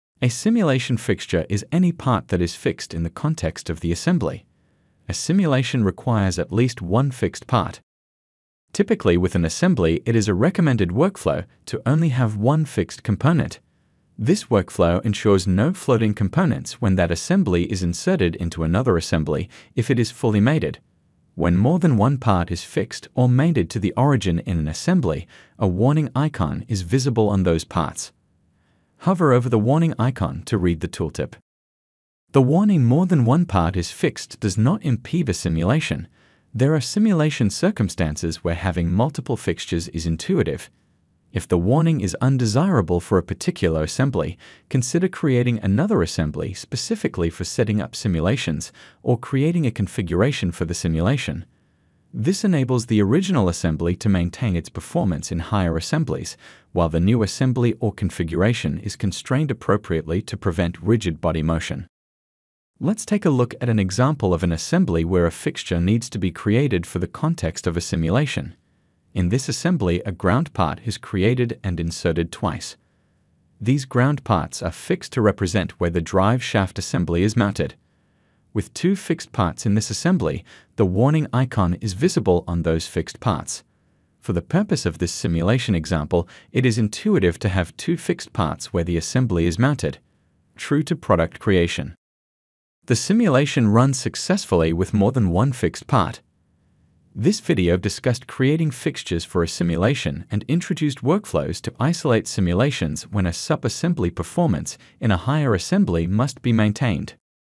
Fixtures in Onshape Simulation
A simulation fixture is any part that is fixed in the context of the Assembly.
A simulation must have at least one fixed part in the Assembly.